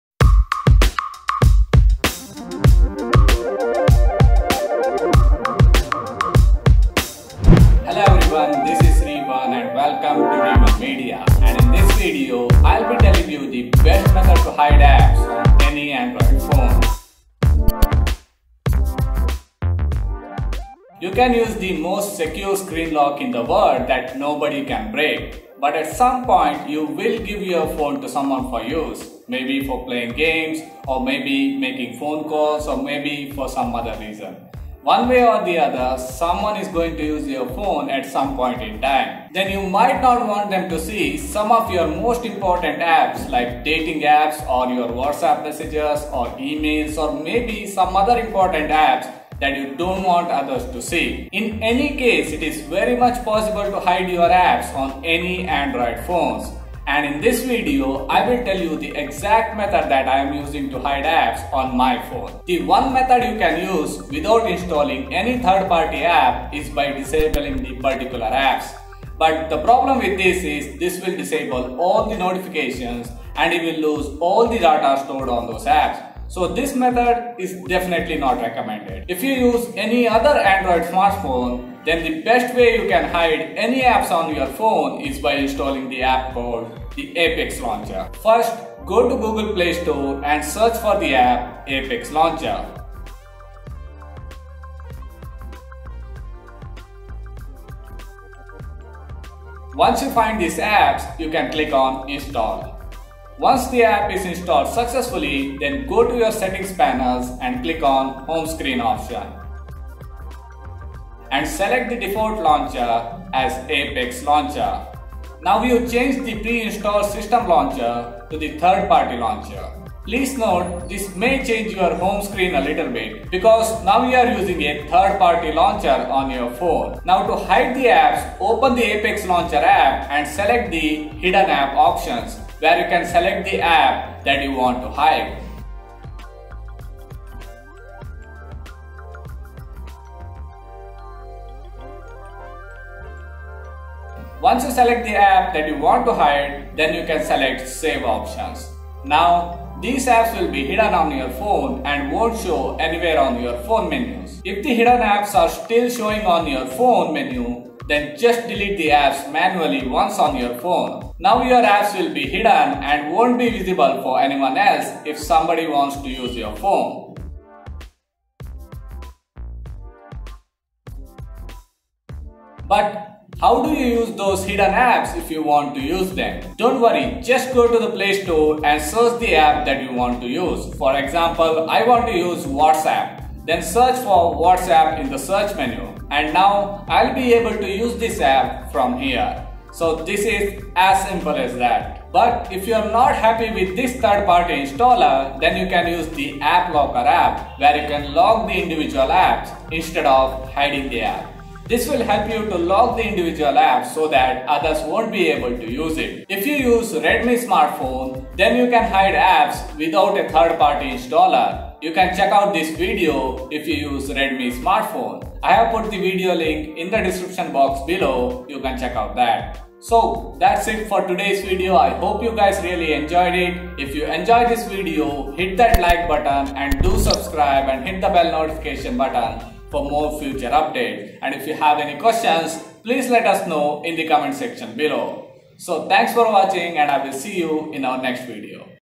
Hide Apps on Any android Phones
In this video, I have made a video about how to hide apps on any android phones.

You can use the most secure screen lock in the world that nobody can break but at some point, you will give your phone to someone for use. Maybe for playing games, or making phone calls or maybe for some other reason. One way or the other someone is going to use your phone at some point in time. Then you might not want them to see some of your most important apps like dating apps or your WhatsApp messages, or your E-mails or maybe some other important apps that you don’t want others to see.

In any case, it’s very much possible to hide your apps on any android phones.  And in this video, I’ll tell you the exact method that I am using to hide apps on my phone.

The one method you can use without installing any third-party apps is ---“by disabling the particular apps”. But the problem with this is this will disable all the notifications and you will lose all the data stored on those apps. So this method is definitely not recommended.

If you use any android smartphone then the best way you can hide any apps on your phone is by Installing an App called Apex Launcher.

First, go to Google Play store and search for the app Apex Launcher. Once you find this app you can click on Install.

Once the app is installed successfully then go to your settings panels and click on Home Screen Option and select the Default Launcher as Apex Launcher.

Now you changed the pre-installed system launcher to third party Launcher.
Please note this may change your home screen a little bit and might show some ads. Because now you are using a third-party launcher on your phone.
Now to hide the apps open the Apex Launcher app and select hidden app options where you can select the app that you want to hide. Once you select the apps that you want to hide then you can select Save option. Now, these apps will be hidden on your phone and won’t show anywhere on your phone menus.

If the hidden apps are still showing on your phone menu then just delete the apps manually once on your phone. Now your apps will be hidden and won’t be visible for anyone else if somebody uses your phone.

But how do you use those hidden apps if you want to use them?
Don’t worry just go to play store and search the app that you want to use for eg. I want to use Whatsapp then ill search for Whatsapp in the search menu and now ill be able to use this app from here. This is as simple as that.

But if you are not happy with this third party installer then you can use the App locker app where you can lock the individual apps Instead of hiding the app, this will help you lock individual apps so that others won’t be able to use it.